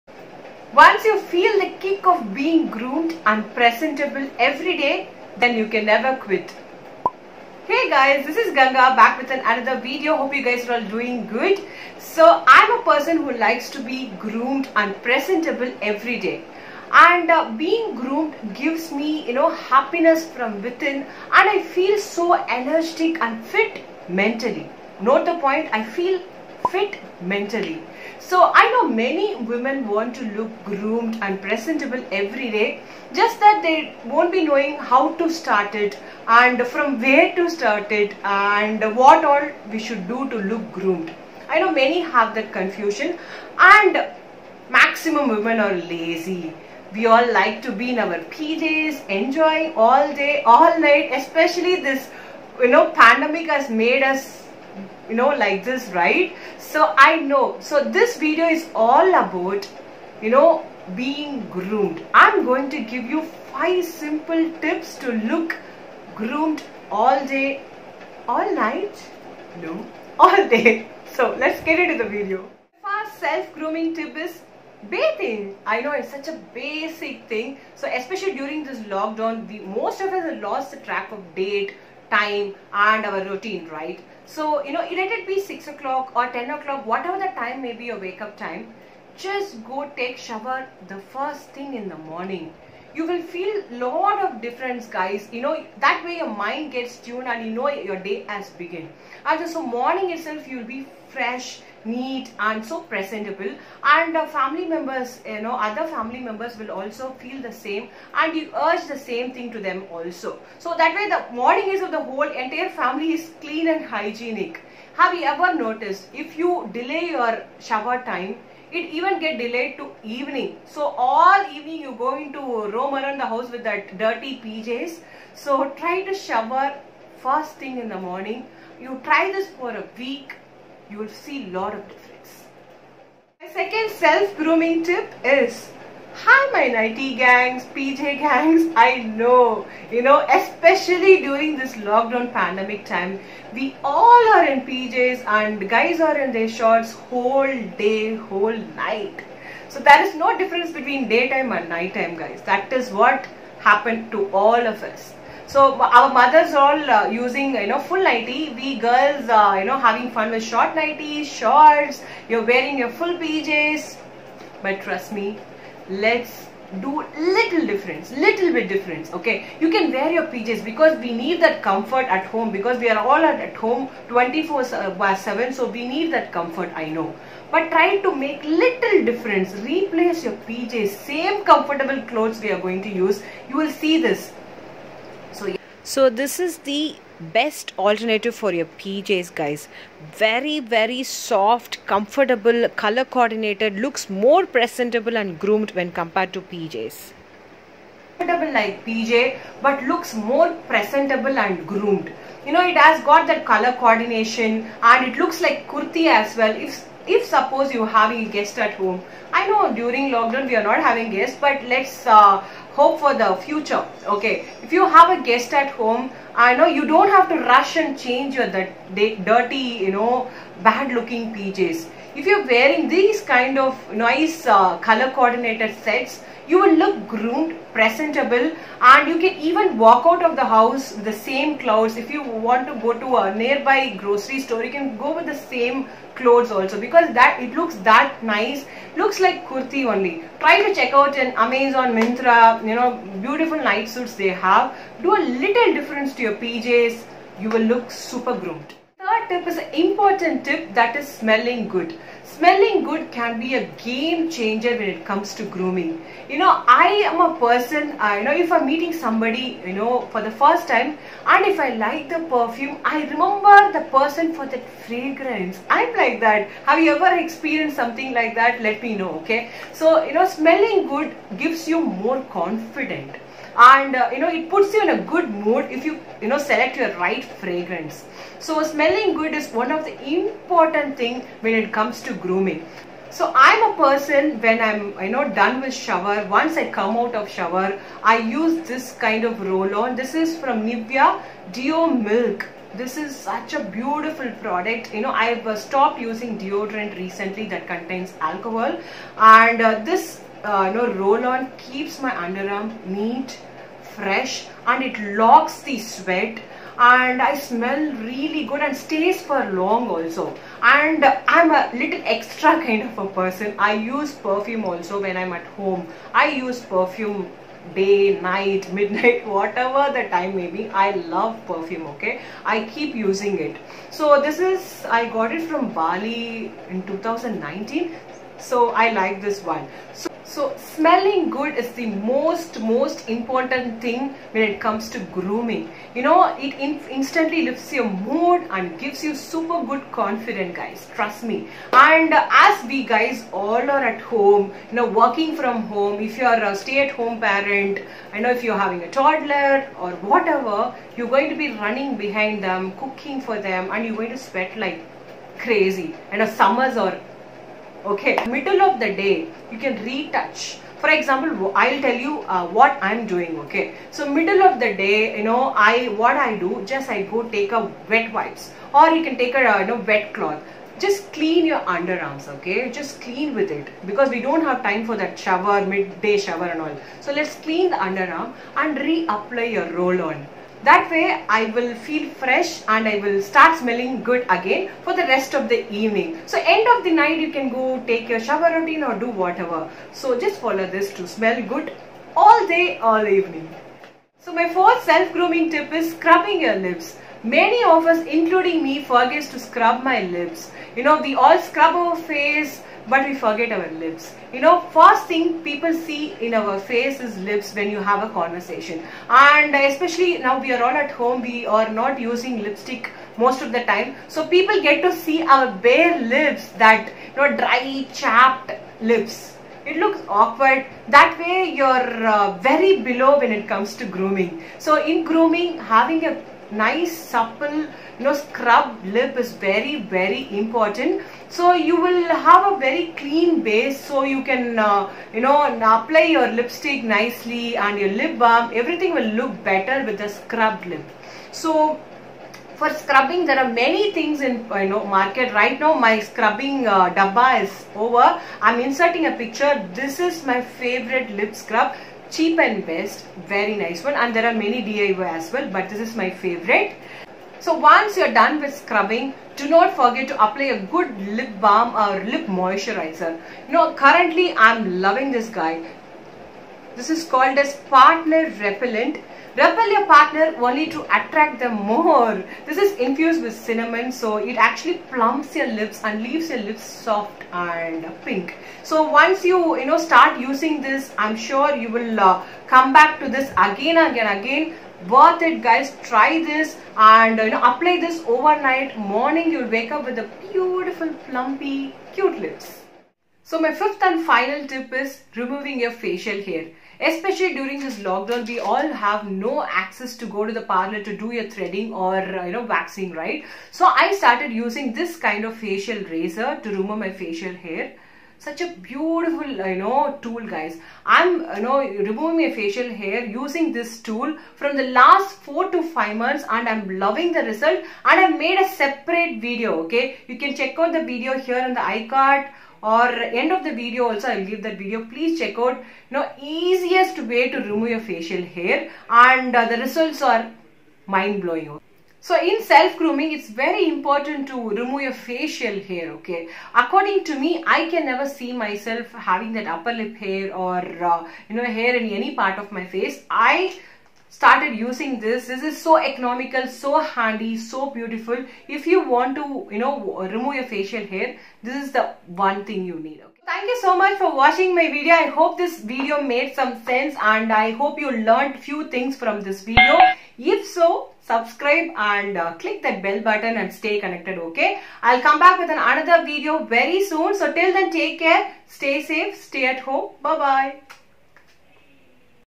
Affordable self grooming tips for women in ENGLISH (2021)/selfgroomingtips
selfgroomingtipsforwomen/affordable selfgrooming tips for women in ENGLISH ( 2021) Groomed,Confident,presantable at home#affordablegroomingtips
This video is all about 5 basic self grooming tips for women which is an affordable self grooming tips for women aswell.
these grooming tips helps you to look confident,presebtable classy everyday
this video helps u to transfer from nighty and pj to more comfortable coord night suit. can easily achieve head to toe grooming by following these grooming tips.
best and affordable self grooming tips for housewife,teenager,working women,nww mothers.